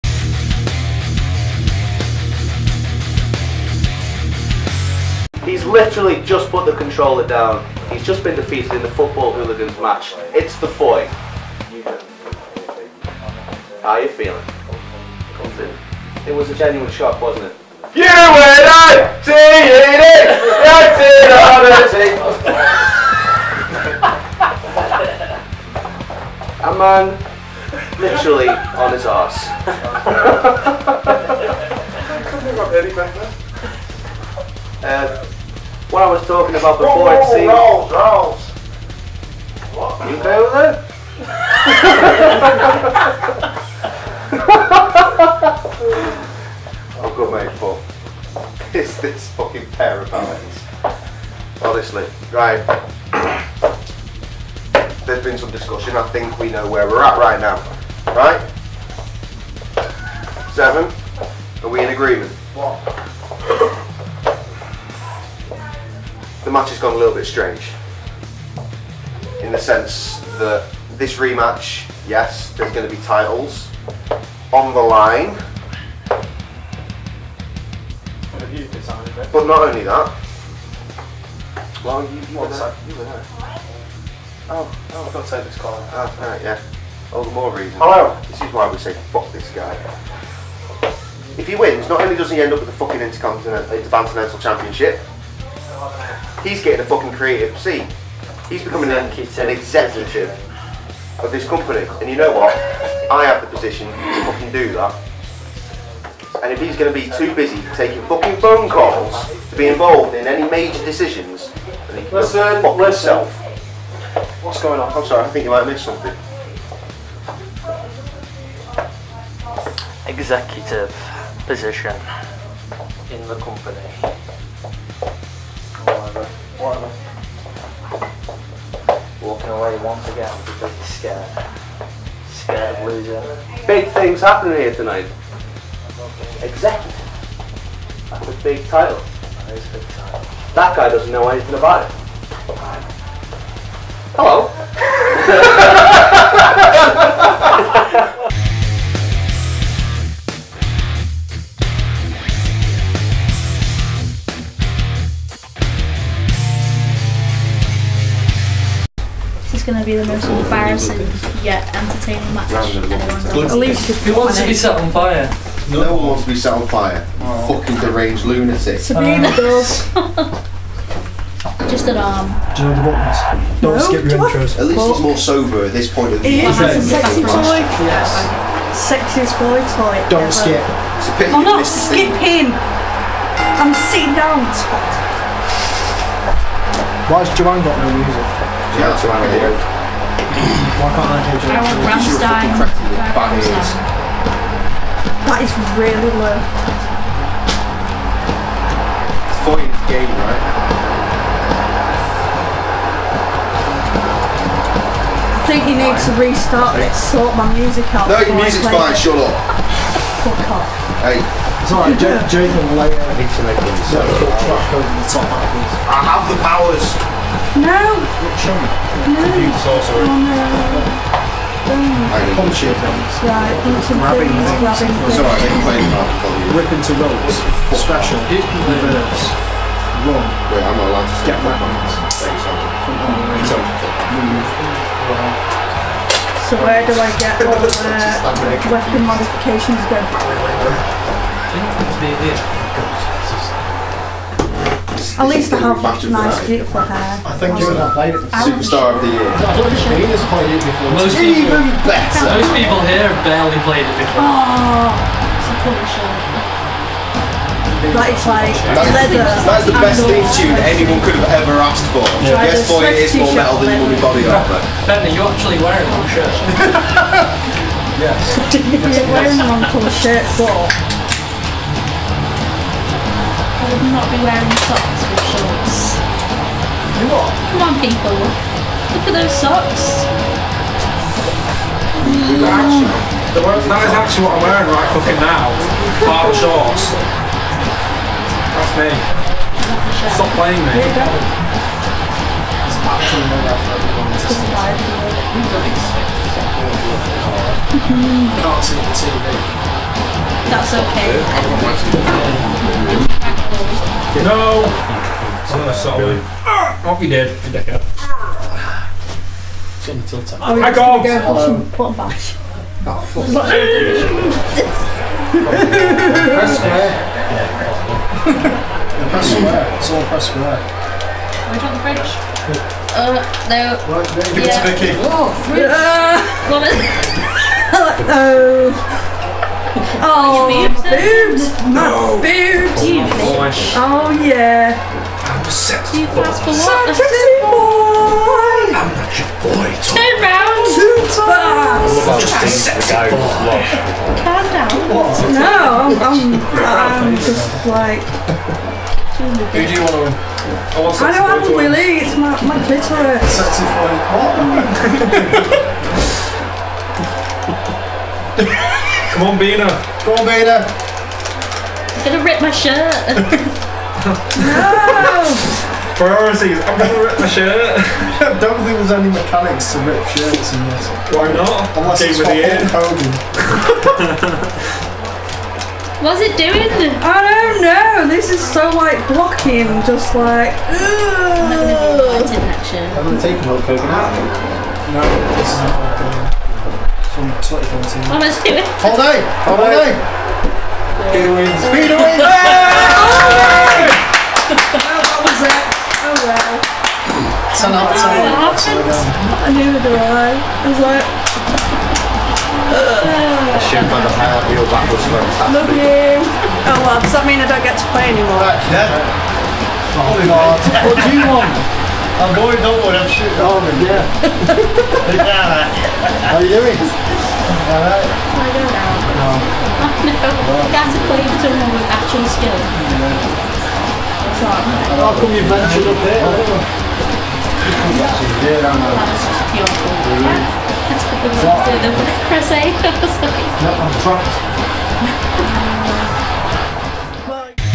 BanterMania 2 #3: Joanne V Jon Bina (Tournament Match)
Day two of BanterMania week kicks off with Tom talking to The Foy with J-Cray-Z, Seven7h Gues7 and Max Heel explaining what happens next with Max's career. We also bring you the Tournament Match between Joanne and Jon Bina!!